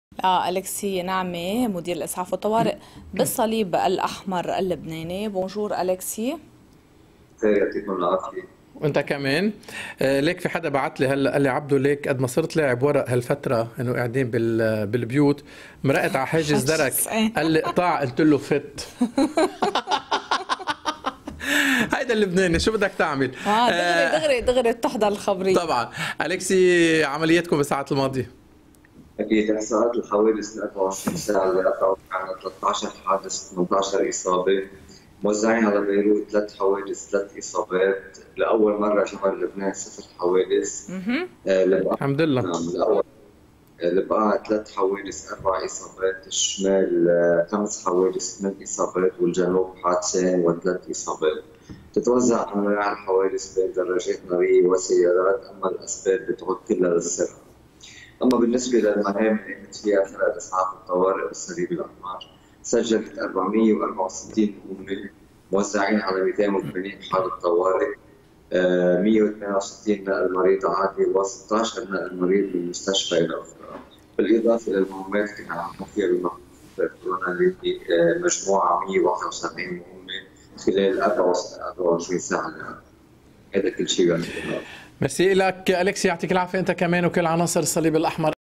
آخر احصاءات وعمليات الصليب الأحمر اللبناني
آخر احصاءات وعمليات الصليب الأحمر اللبناني مع مدير الاسعاف والطوارئ في الصليب الاحمر الكسي نعمة